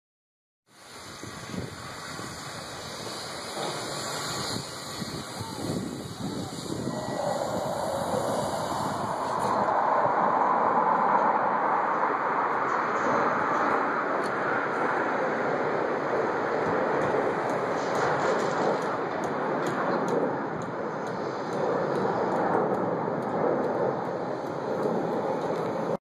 Blue Angels Practice
The US Navy's Blue Angels practice before Seattle's SeaFair weekend near Lake Washington and the University of Washington Campus.